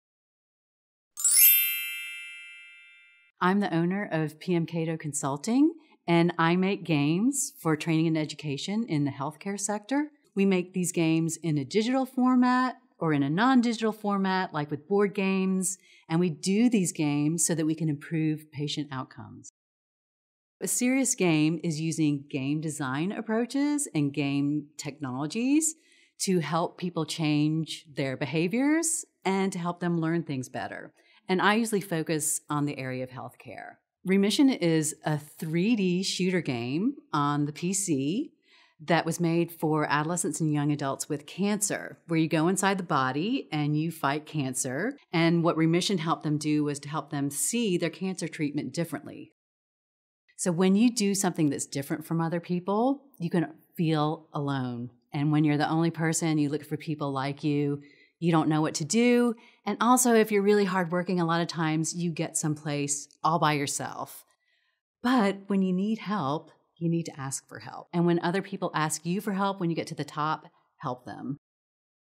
Social Entrepreneur | Pamela Kato | 60 Seconds
Watch Pamela Kato, owner of P.M. Kato Consulting. Her company makes digital and non-digital games for training and education in the healthcare sector.